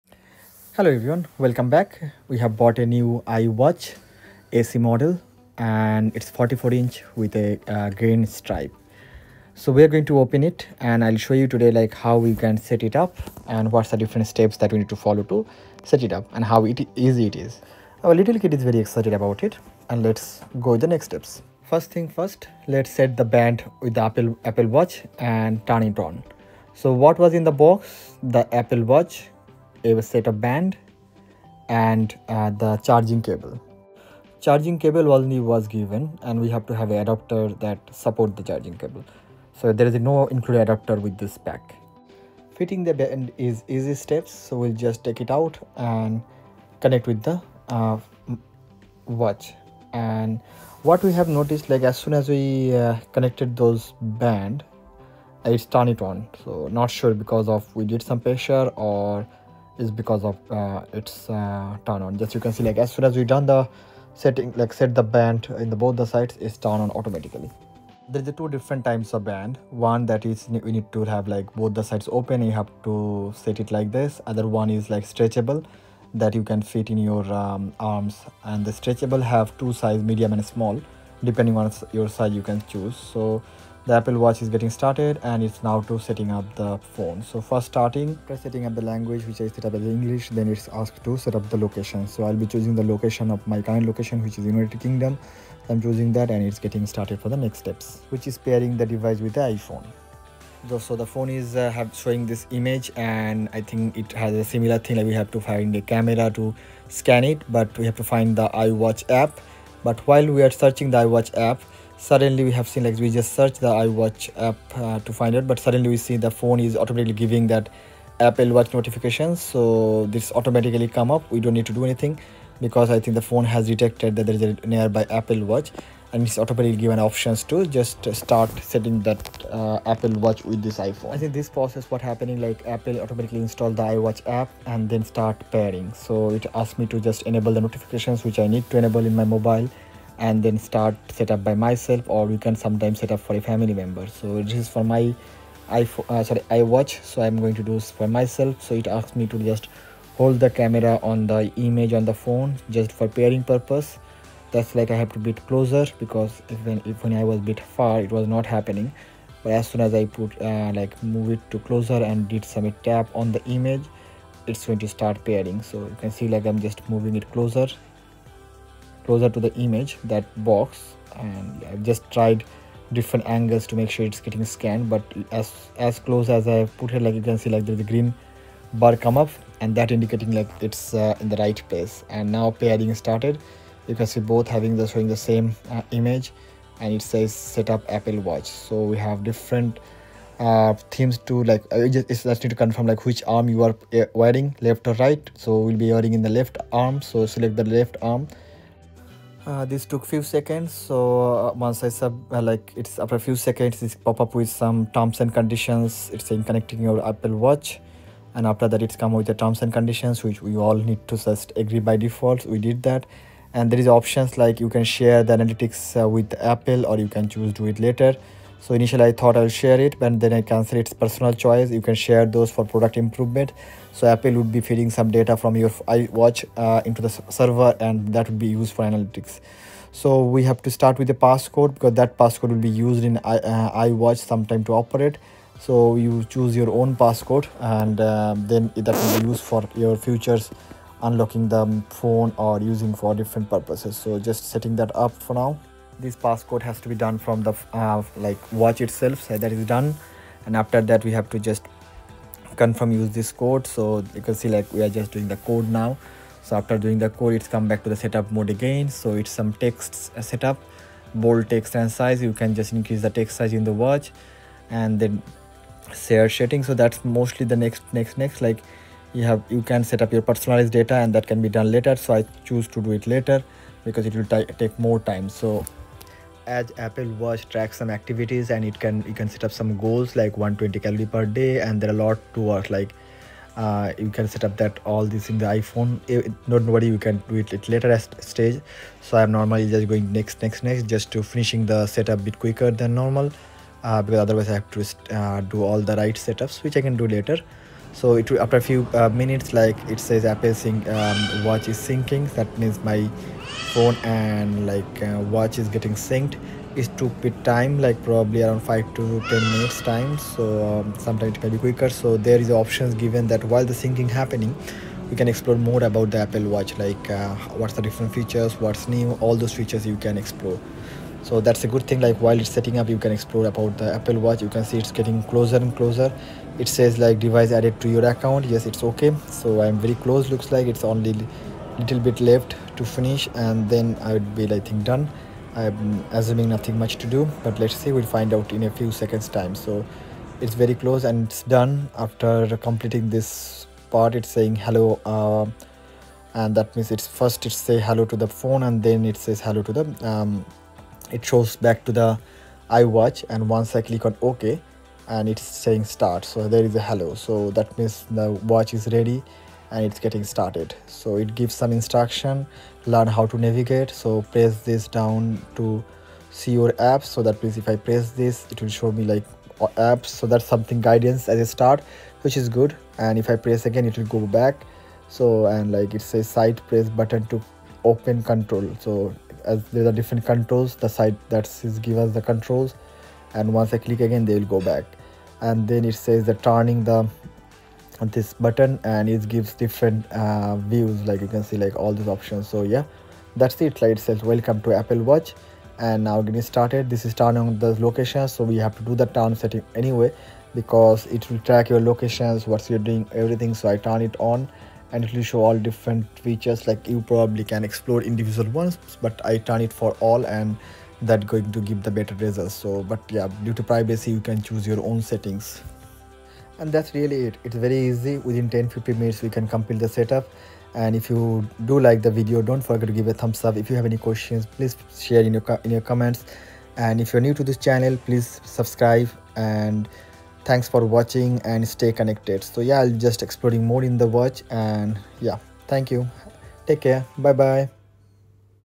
How to setup Apple Watch | Step by Step Apple Watch Setup Guide
🕰️ How to Set Up Apple Watch | Step-by-Step Apple Watch Setup Guide ⌚📱

Just got a new Apple Watch? 🎉 Whether it’s your first time setting it up or you need a refresher, this easy step-by-step guide will help you get your Apple Watch up and running in no time! ⌚✅

📌 In this video, you’ll learn:
🔹 How to pair your Apple Watch with iPhone 📱
🔹 Complete all setup upto start of the Watch

This guide works for Apple Watch Series 9, Ultra 2, SE, and older models running watchOS.